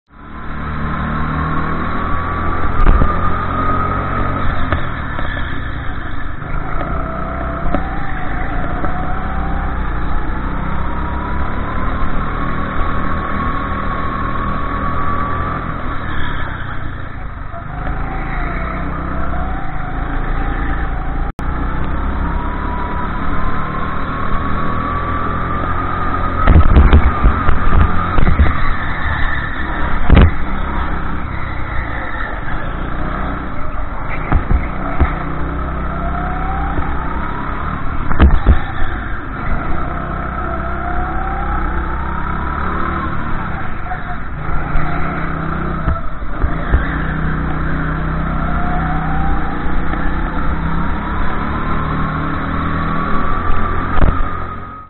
Go-Karting - London Docklands new track! (multi-level 800m)
A lap round the new and improved indoor track at London Docklands.